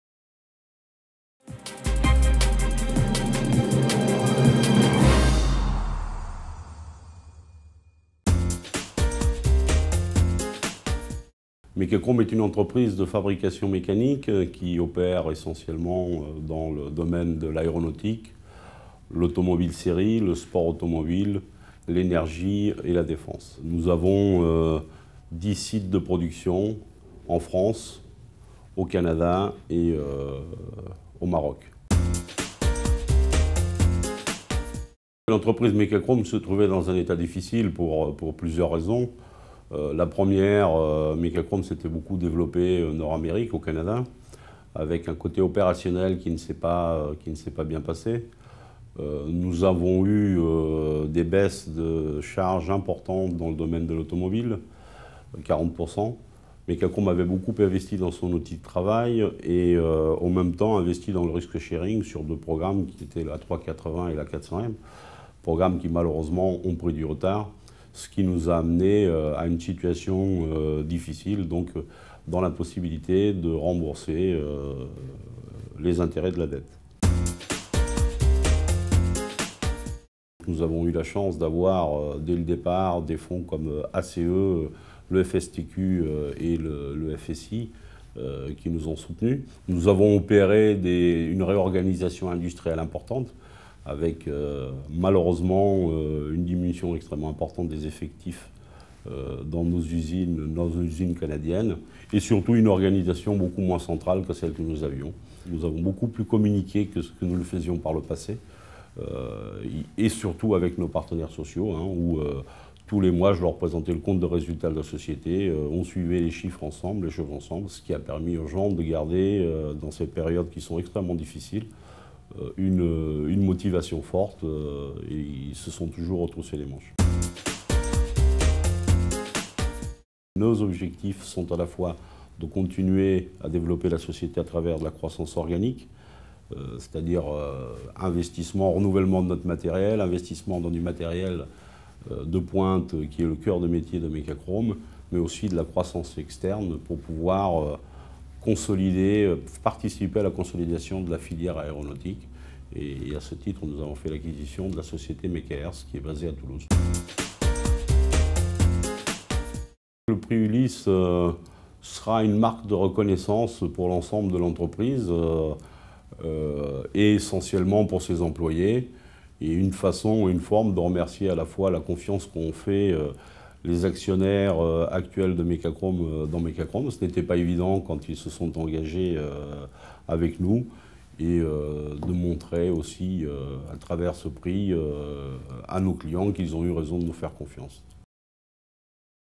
Prix Ulysse 2012 - entreprise MECACHROME
MECACHROME, vainqueur du Prix Ulysse 2012 récompensant le meilleur retournement d'entreprise.